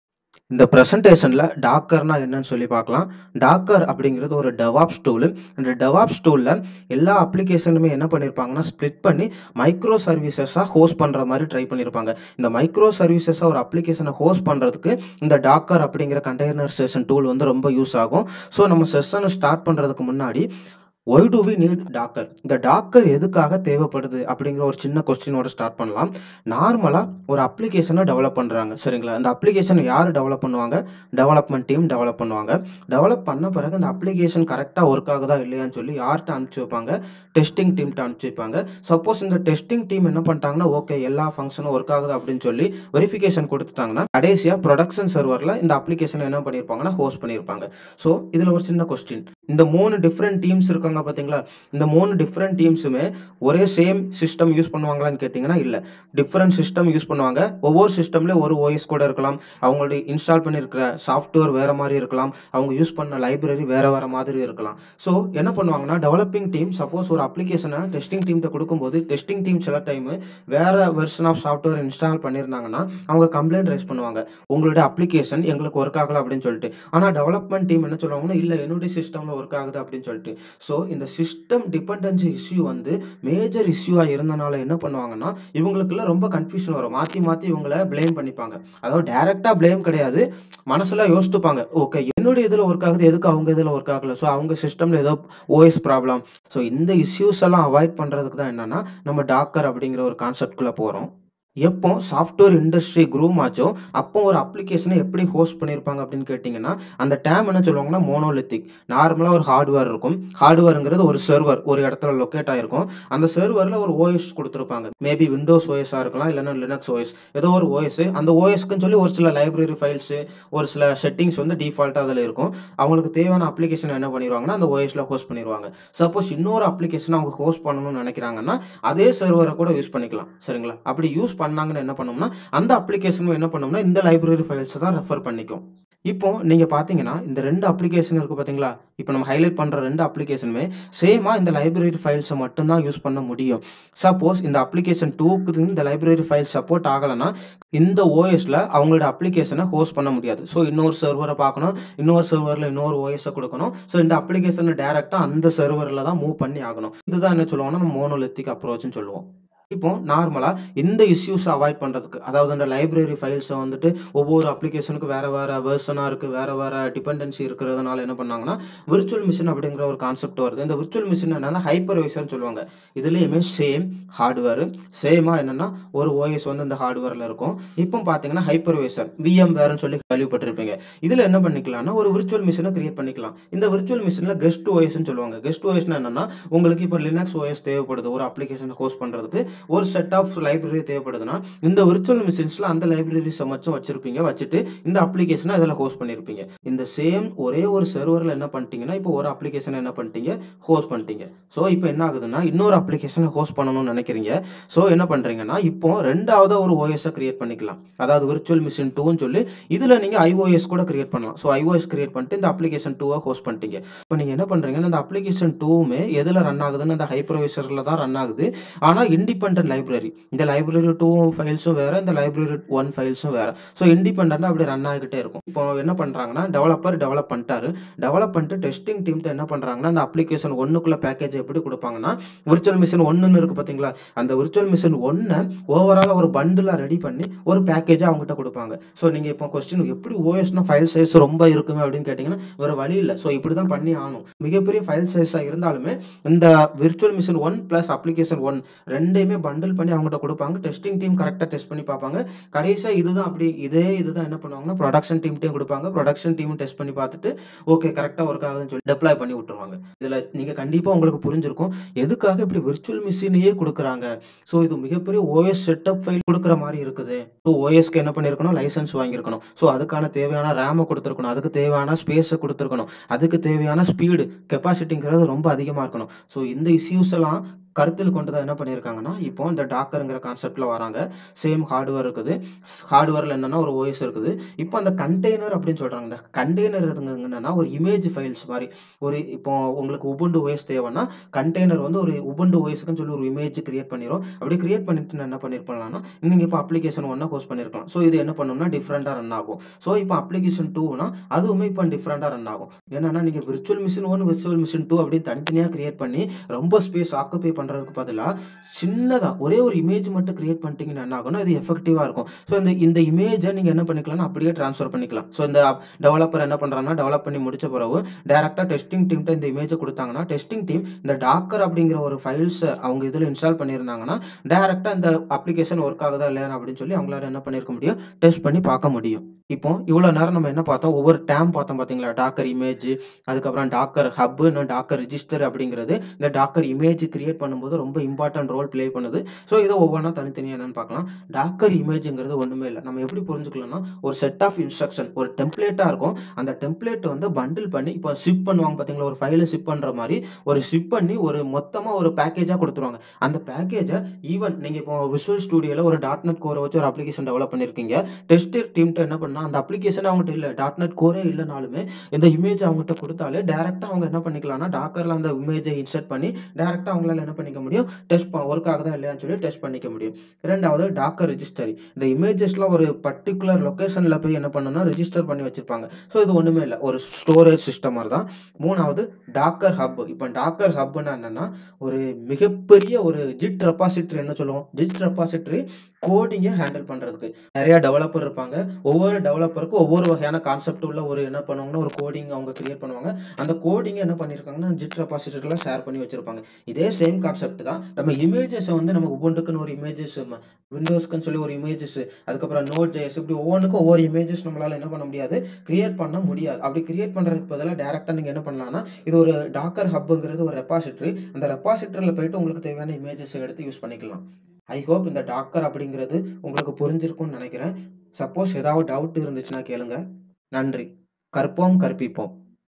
What is docker | Containerization | Introduction | Tamil
Docker use OS level virtualization to provide software in packages called containers

what's is docker?
what is docker image?
what is docker registry?
what is docker hub?
why we choose docker instead virtual machine?
advantages of docker